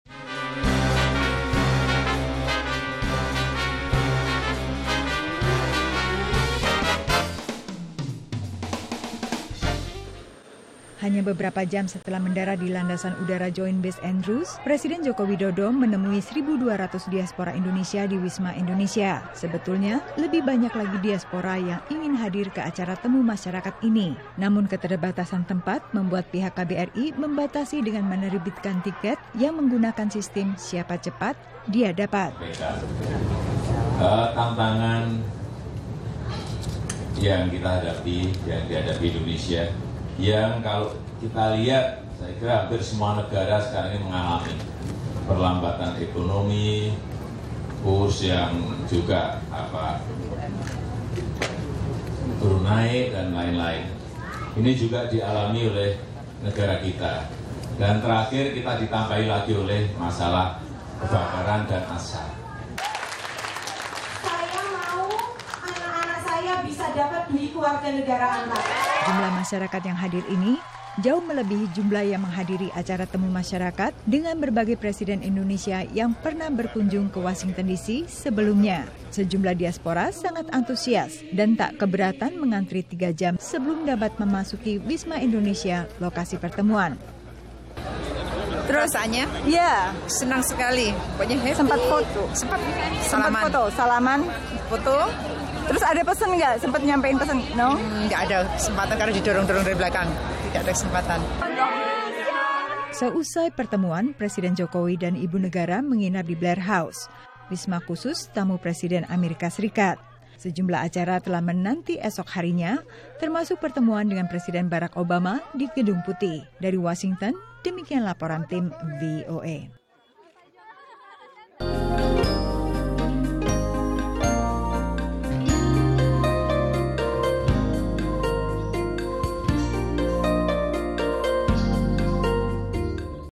Tatap Muka Jokowi dengan Warga Indonesia di Washington DC
Presiden Jokowi bertatap muka dengan warga Indonesia di Washington DC dalam kunjungan hari pertama di Amerika Serikat. Dalam kesempatan ini Jokowi menjelaskan tantangan-tantangan yang dihadapi Indonesian dan upaya-upaya pembangunan ekonomi yang dilancarkan pemerintahannya.

Posted By : Lensa Jakarta
News Source : VOA